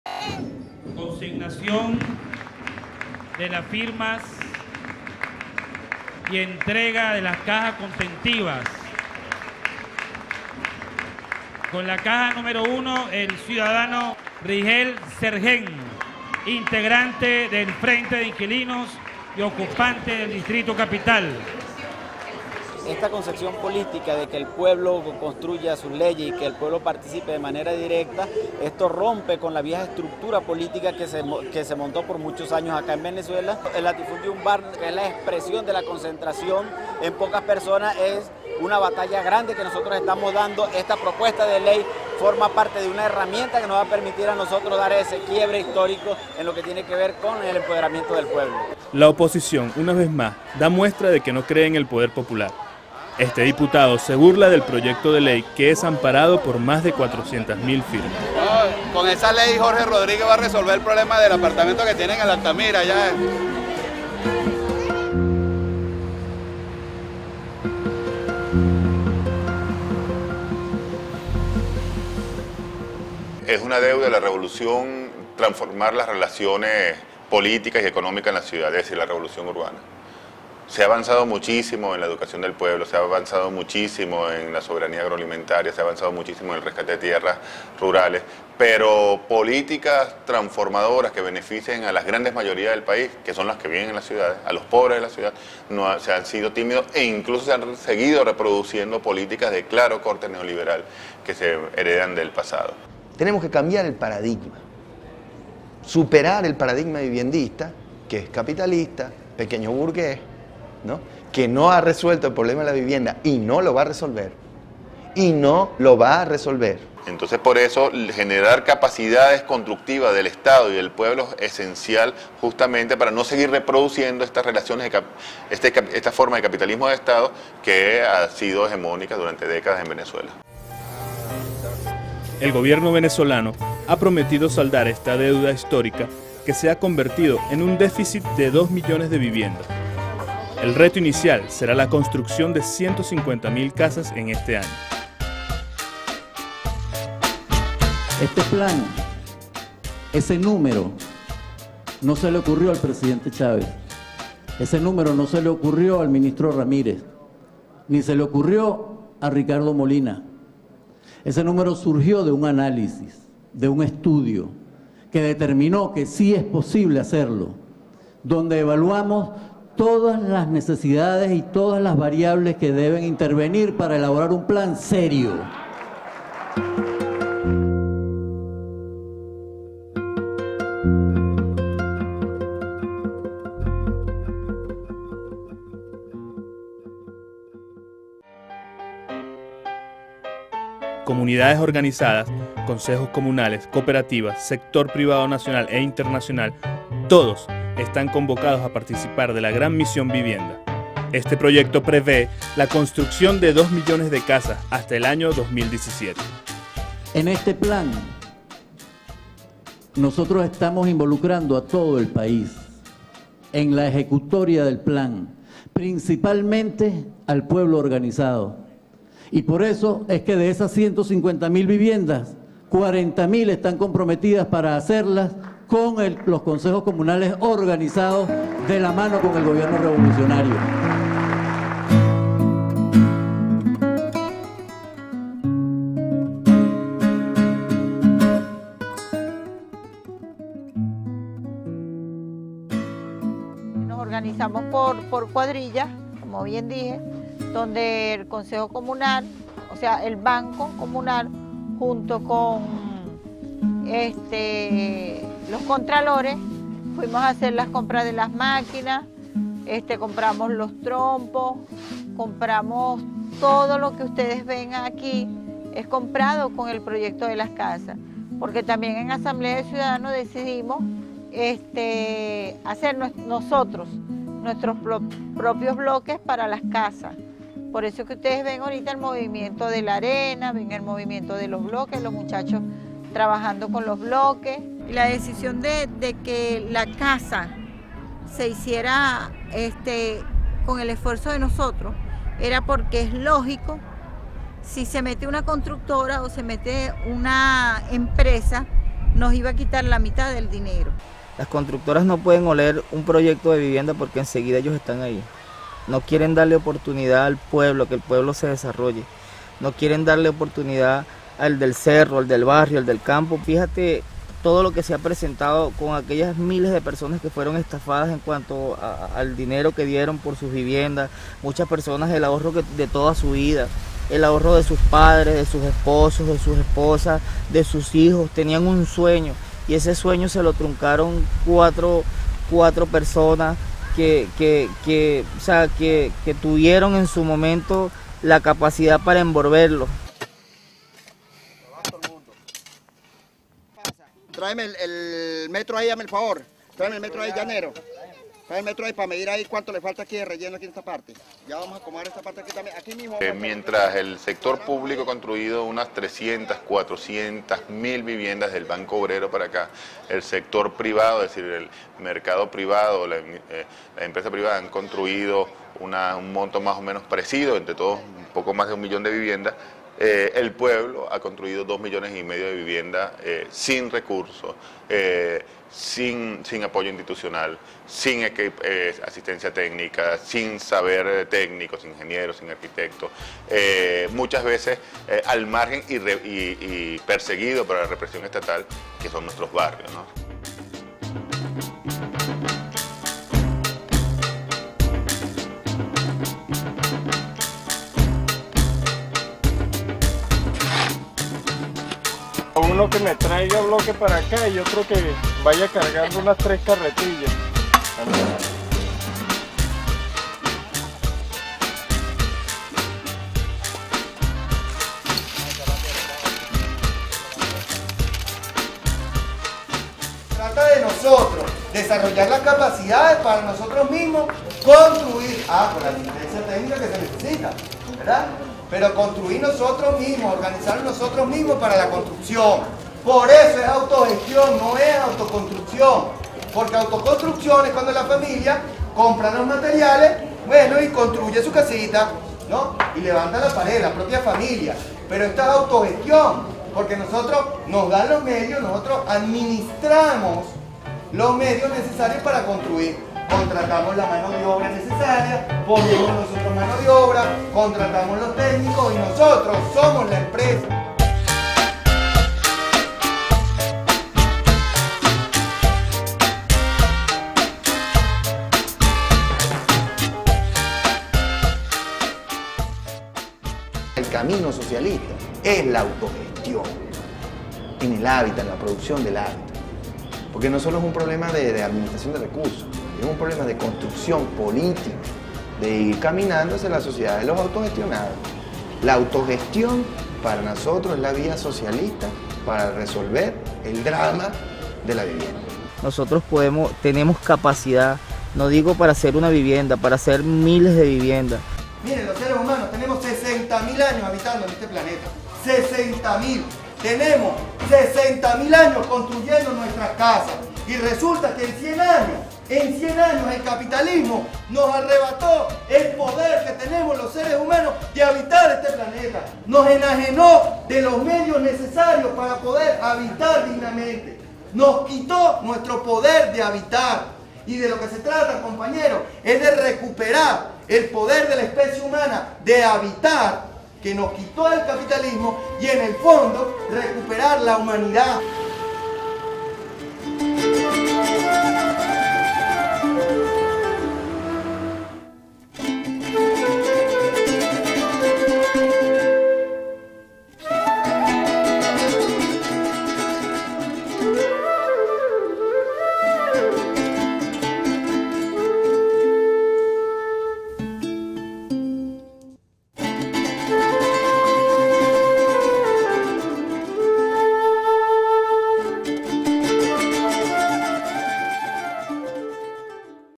Crisis Habitacional, el poder popular contra el latifundio urbano - parte 2
El modelo de desarrolo urbano en Caracas impidió a millones de excluidos el poder habitar una vivienda digna. El Poder Popular junto al gobierno revolucionario se organiza y lucha hoy para acabar con el latifundio urbano.